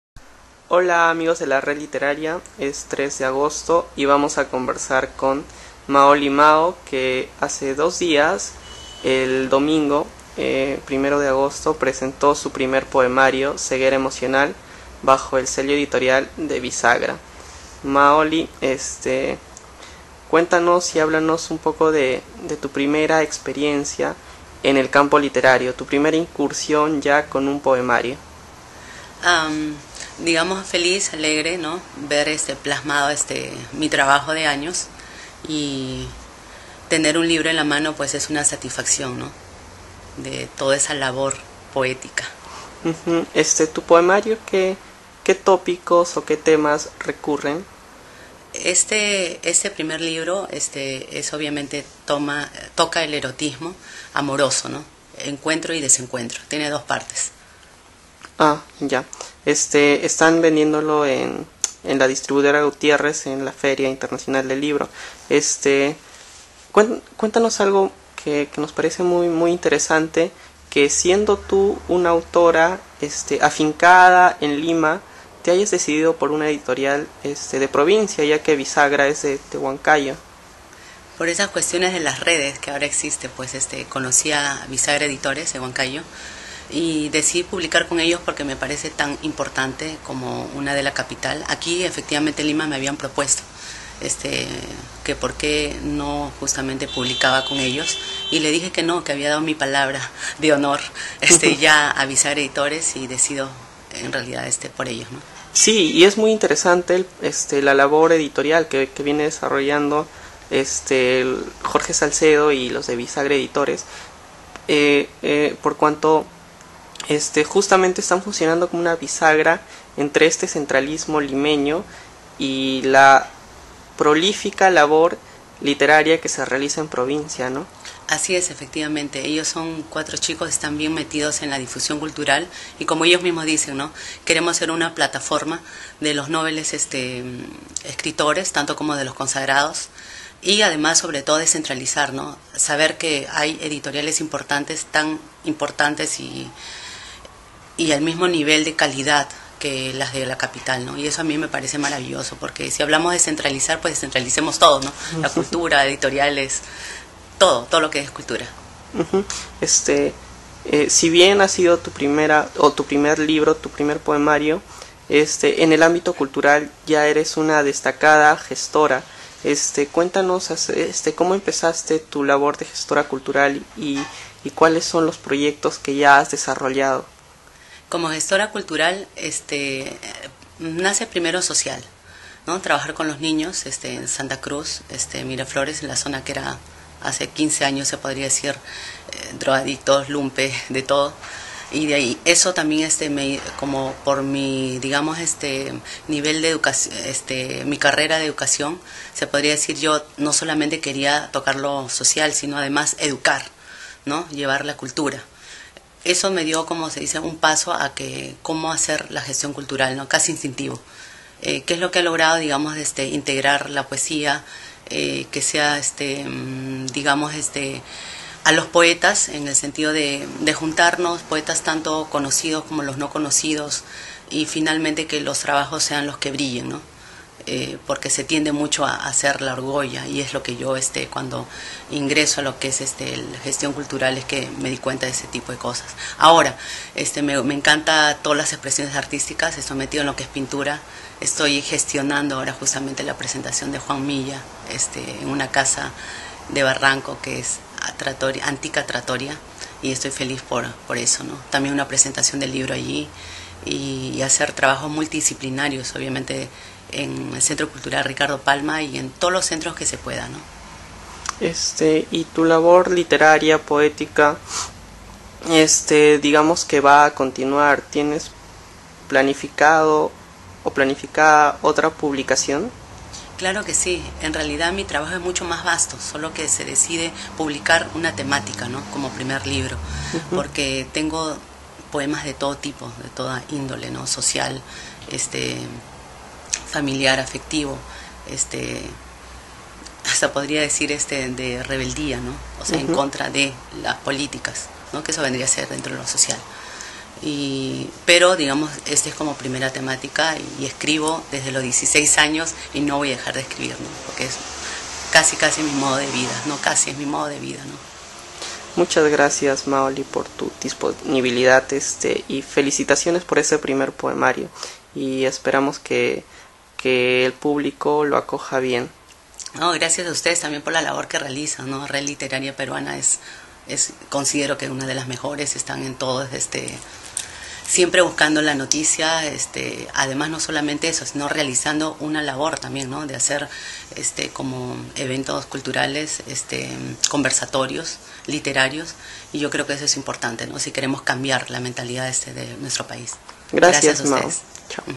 Entrevista a la poeta Maoli Mao
Maoli Mao, poeta y gestora cultural, presentó en la FIL 2010 su primer poemario "Ceguera emocional".